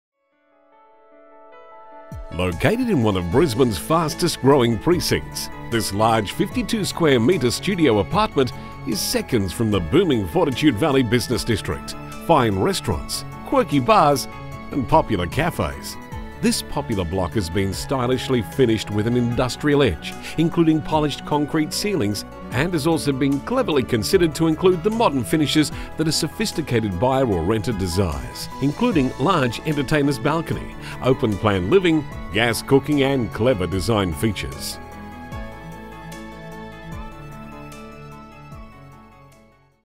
206/82 Alfred Street - Fortitude Valley Queensland by Brando...
Located in one of Brisbane's fastest growing precincts, this large, 52sqm studio apartment is seconds to the booming Fortitude Valley business district, fine restaurants, quirky bars and popular cafes. This popular block has been stylishly finished with an industrial edge including polished concrete ceilings and has also been cleverly considered to include the modern finishes that the sophisticated buyer or renter desires, including large entertainer's balcony, open plan living, gas cooking and clever design features.
206/82 Alfred Street, Fortitude Valley